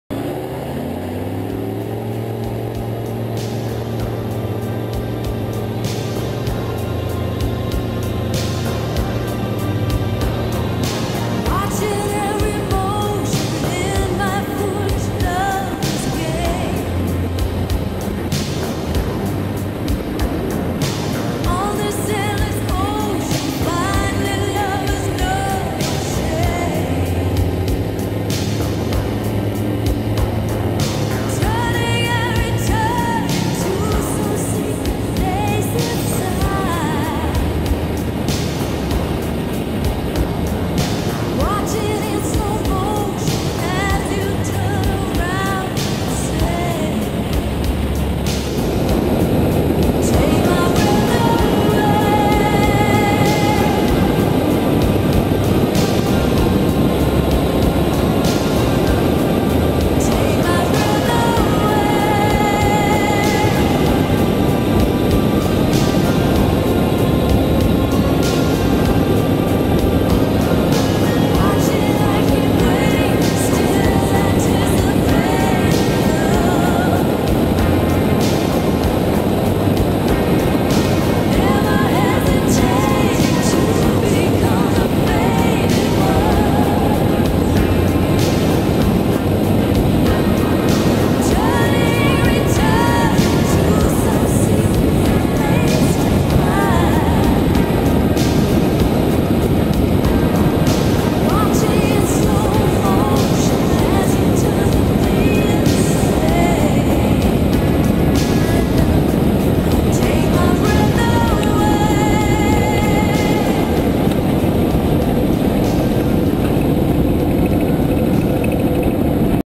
Rainbow Cheetah - SSG Quick view
This video is purely for evaluation of the wing mounted camera's capture.
A few random clips of Steve in his Rainbow Cheetah (Busgcat) SSG, doing some aerial photography work around Durban.